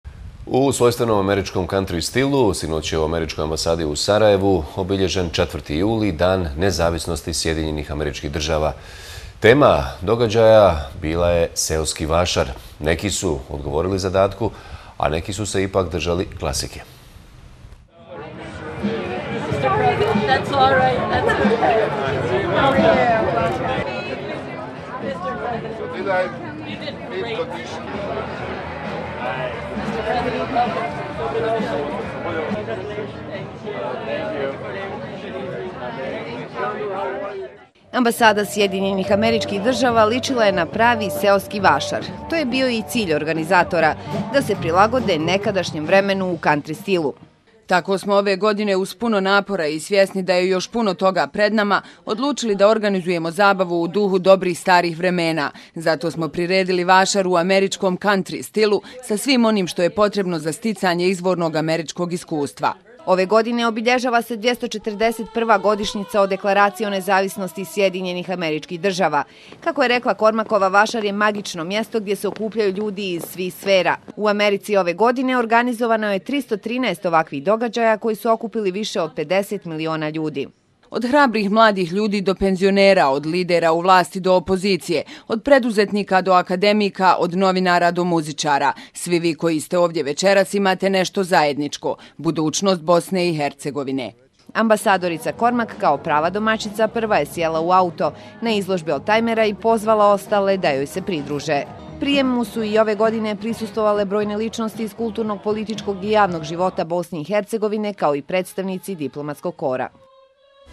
U ambasadi SAD obilježen Dan nezavisnosti
U svojstvenom američkom kantri stilu, obilježen je 4. juli Dan nezavisnosti SAD u Američkoj ambasadi u Sarajevu. Tema događaja bila je seoski vašar. Neki su odgovorili zadatku, a neki su se ipak držali klasike.

Ambasada SAD ličila je na pravi seoski vašar. To je i bio cilj organizatora. Da se prilagode nekadašnjem vremenu u kantri stilu.

"Tako smo ove godine, uz puno napora i svjesni da je još puno toga pred nama, odlučili da organizujemo zabavu u duhu dobrih starih vremena. Zato smo priredili vašar u američkom, kantri stilu, sa svim onim što je potrebno za sticanje izvornog američkog iskustva", poručila je ambasadorica SAD u BiH Morin Kormak.

Ove godine obilježava se 241.godišnjica deklaracije o nezavisnosti SAD-a. Kako je rekla Kormakova, vašar je magično mjesto gdje se okupljaju ljudi iz svih sfera. U Americi ove godine organizovano je 313 ovakvih događaja koji su okupili više od 50 miliona ljudi.

- Od hrabrih mladih ljudi do penzionera, od lidera u vlasti do opozicije, od preduzetnika do akademika, od novinara do muzičara. Svi vi koji ste ovdje večeras imate nešto zajedničko - budućnost BiH, poručila je Kormakova.

Gosti su mogli pogledati izložbu oldtajmera, slikati se u "porodičnoj galeriji portreta" ili oprobati se u frizbiju, američkom fudbalu i nekim drugim zabavnim igrama. Ambasadorica Kormak kao prava domaćica prva je sjela u auto, na izložbi oldtajmera i pozvala ostale da joj se pridruže. Prijemu su i ove  godine prisustvovale brojne ličnosti iz kulturnog, političkog i javnog života Bosne i Hercegovine, kao i predstavnici diplomatskog kora.